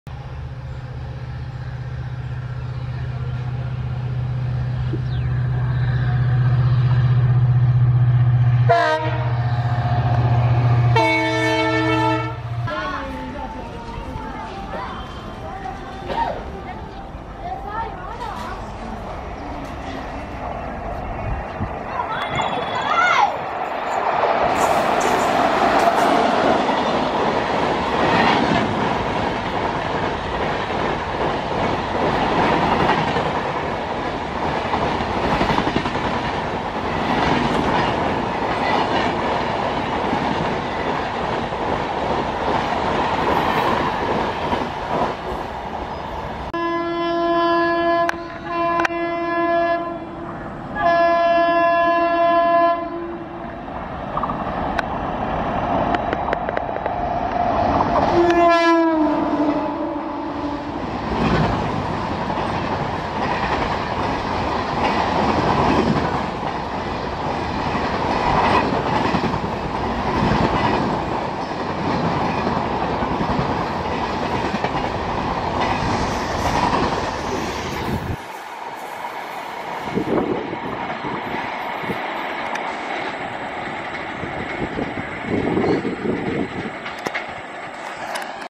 12759 Charminar Express at MPS in Chennai outskirts | Indian railways
12759/Charminar Express  departs from Tambaram  at 17:10  and covers  818 KM  within  14h 40m hrs with average speed of 56   kmph

Location :Palavanthangal, Minambakkam

camera used Cannon sx 70HS